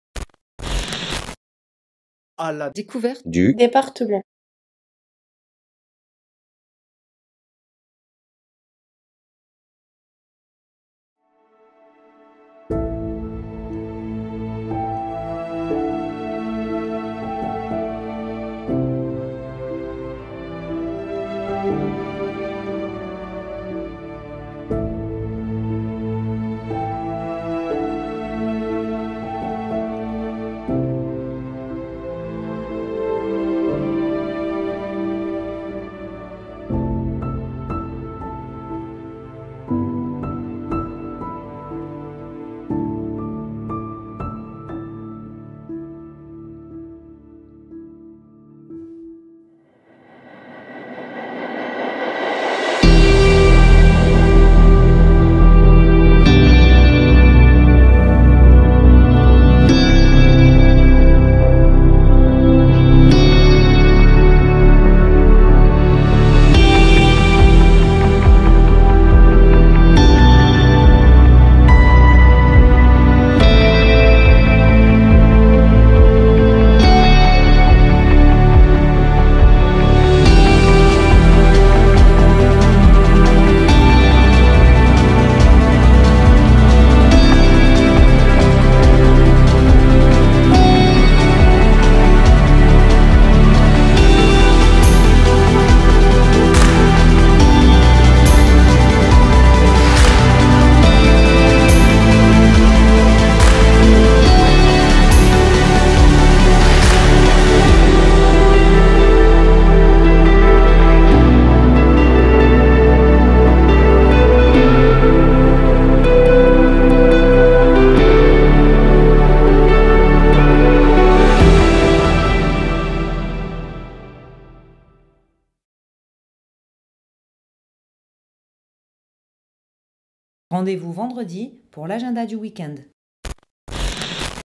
Le Verdon vu du ciel
Voici un voyage en deltaplane motorisé au dessus du Verdon !